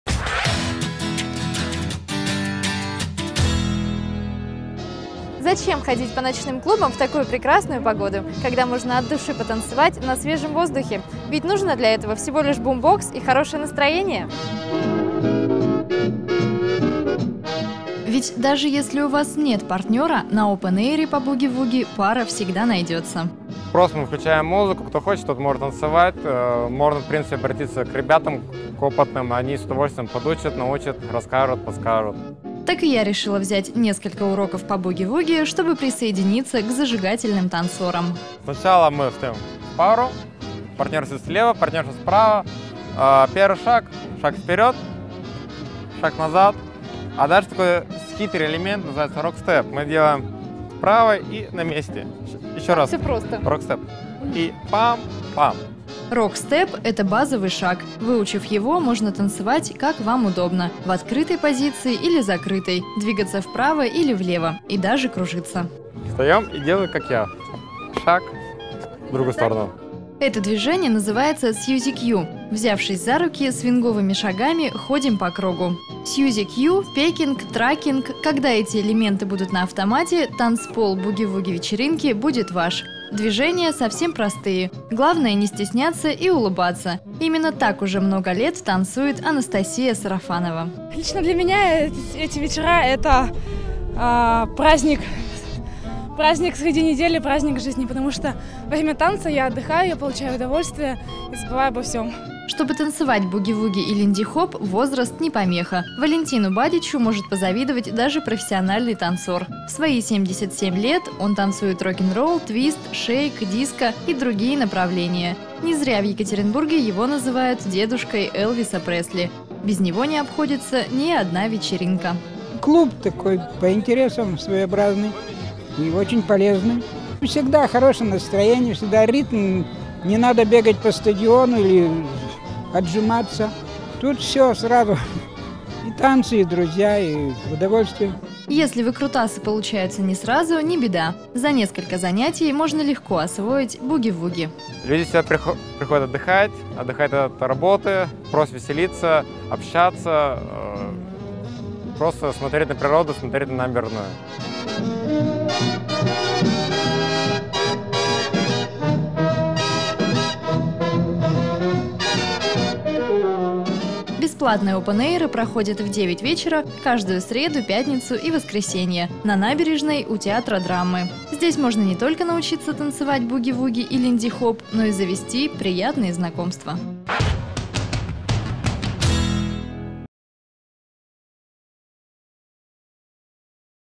Активный отдых: танцуем буги-вуги!
Для того чтобы потанцевать от души, нужны только бумбокс, набережная и отличная компания! Танцуем буги-вуги!